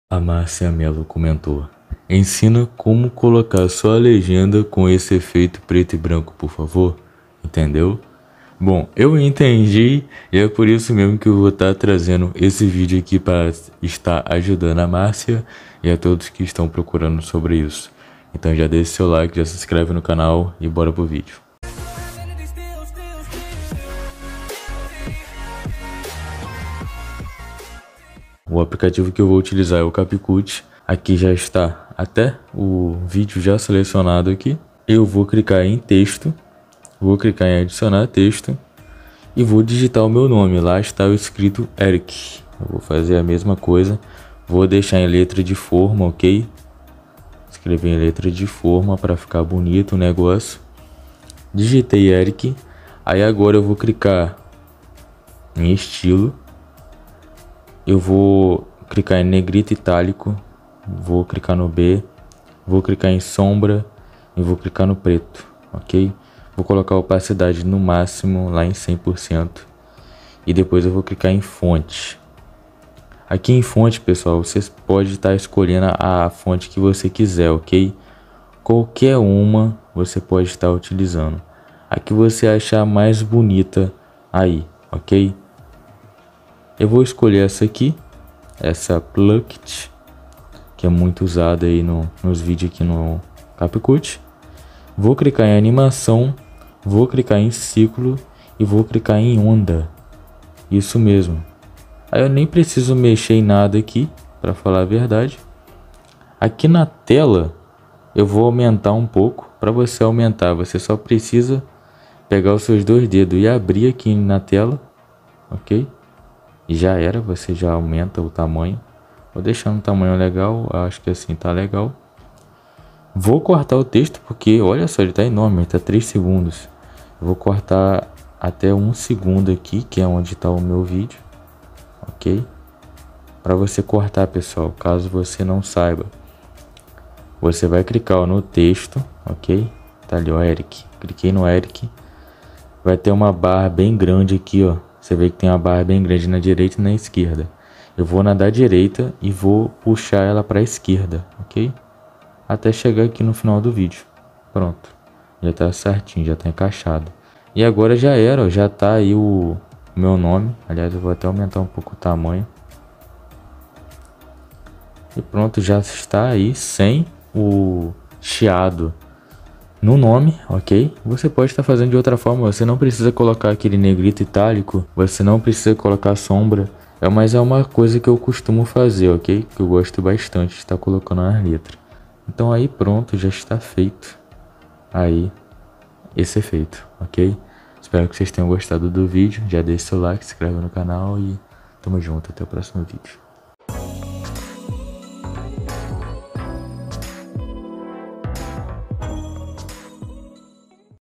Como colocar LEGENDA COM EFEITO DE CHIADO PARA SEU LOMOTIF - parecido com lomotif de video star*
MARCIA MELO COMENTOU '' ENSINA COMO COLOCAR SÓ A LEGENDA COM ESSE EFEITO PRETO E BRANCO PFV? ENTENDEU? '', COM ISSO EU FIZ ESSE VÍDEO PARA AJUDAR NÃO SÓ A ELA, MAS A TODOS VOCÊS.

o efeito de chiado é um efeito muito usado nos lomotifs, com isso eu fiz aquele primeiro vídeo, e teve uma dúvida de uma inscrita, com isso eu fiz esse vídeo para ensinar a colocar o efeito de chiado com uma legenda com seu nome na frente do efeito.